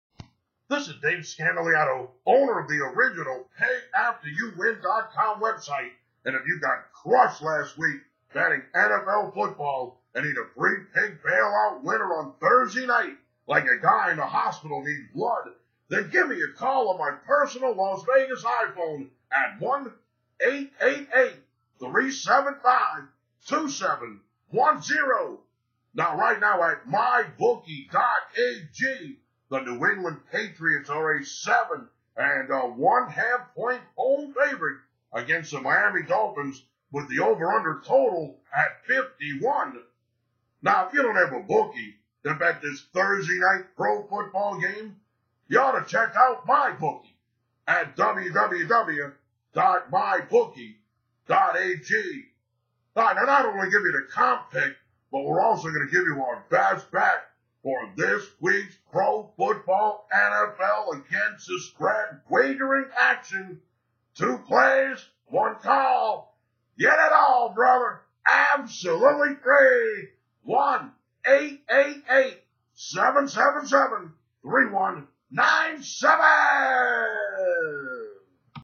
Free Pick New England Patriots vs Miami Dolphins NFL Bet Preview Prediction Thursday October 29 2015
website as he talks about the week #8 NFL pro football contest between the New England Patriots versus the Miami Dolphins on Thursday October 29th 2015.

To talk with Dave Scandaliato directly please call toll free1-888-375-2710 from now up until Thursday 10-29-15.